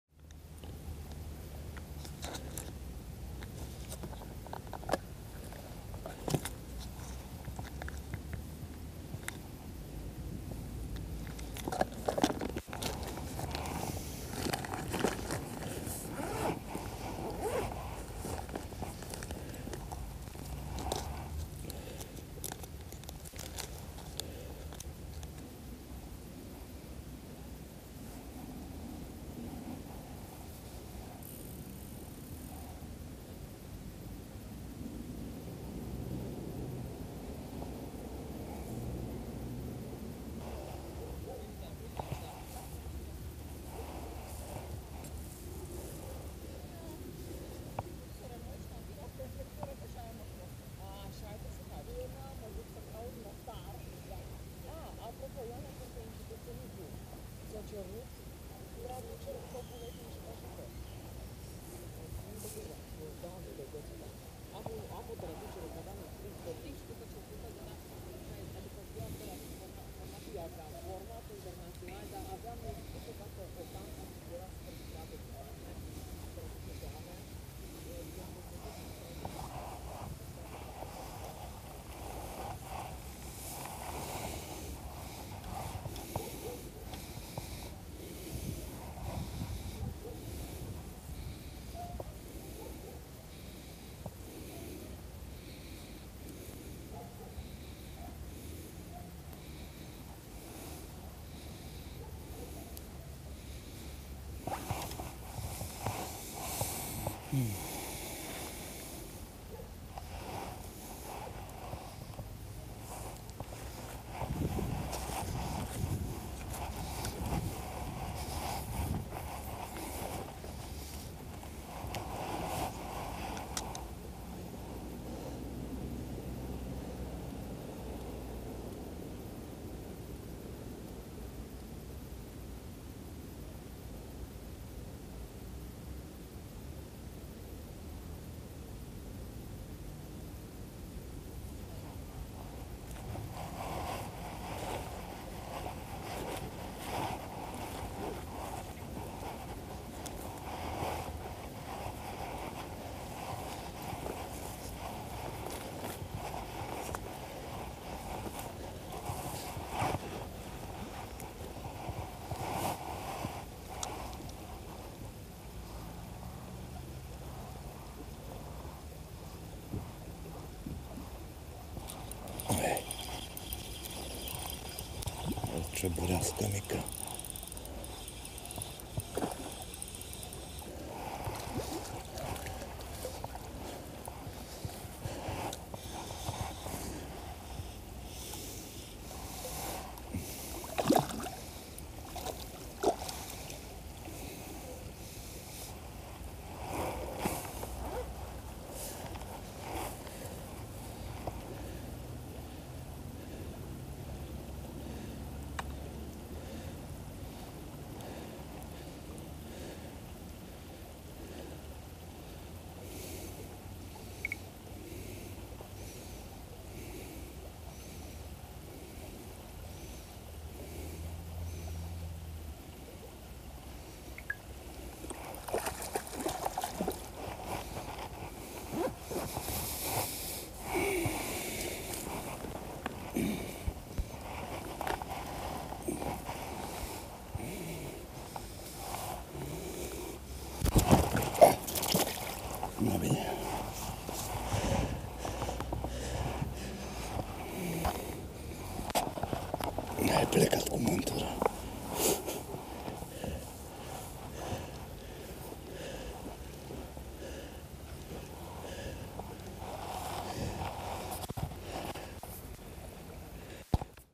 Paraul Peta si surprizele ei
Surpriza placuta pe Peta azi.
Micro offset + micro cheburashka de 1g + grub curly tail Manns
pe apa pana la glezne
Bat Rapture Drone BTX Bottom 2.10 de 0-4g
Mulineta Daiwa Legalis LT 1000 XH
Fir Delphin Troutera 0.04 de 4lbs